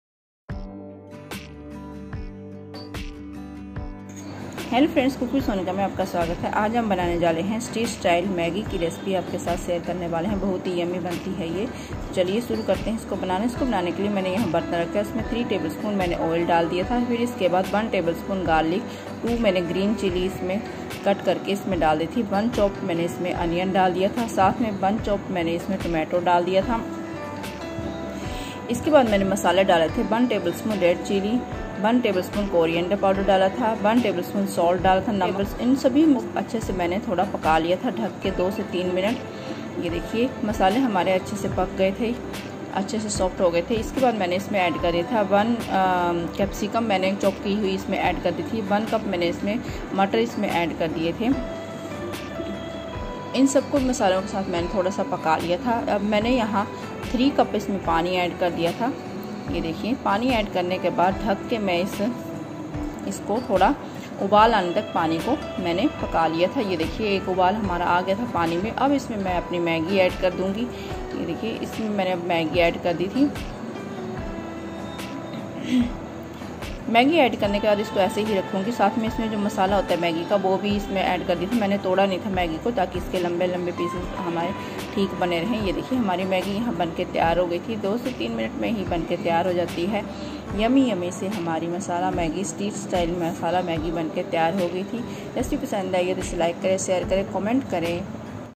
Masala Maggi Recipe/How to make Maggi masala/एक बार Maggi ऐसे बना कर तो देखे /Veg maggi Recipe
Ingredients

Maggi
Onion 1
Tomatoes 2
Red chilli 1 tsp
Coriander poweder 1 tsp
Salt 1 tsp
Capsicup 1 cup